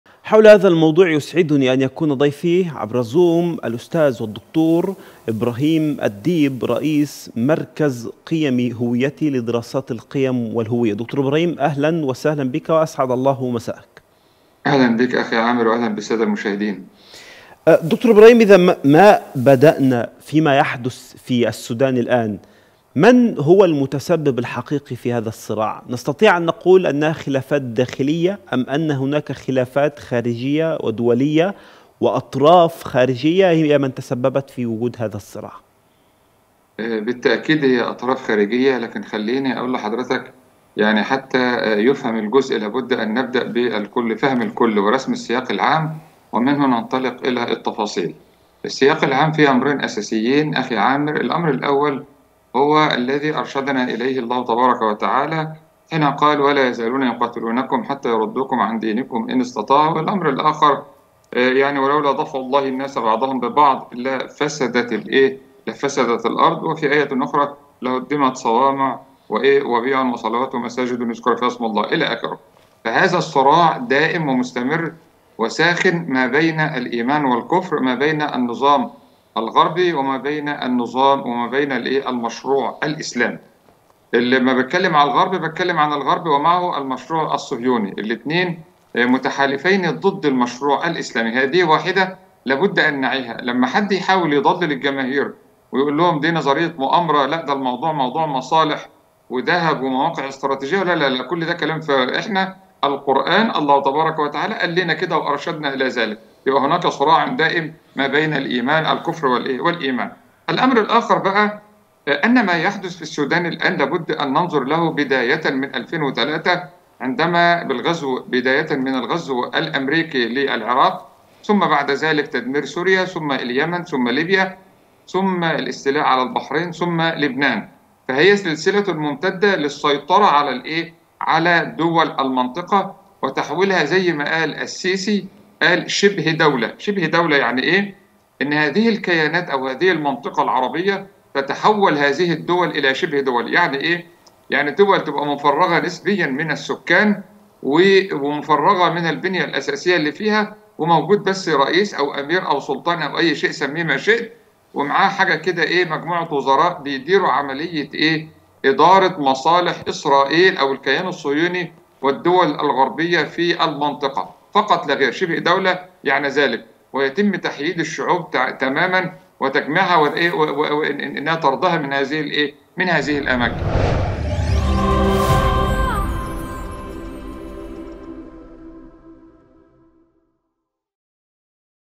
الحرب الكبيرة التي لم يراها السودانيون بعد ! أمريكا وروسيا وهذه الدول العربية
Nilesat (النايل سات): 11603 أفقي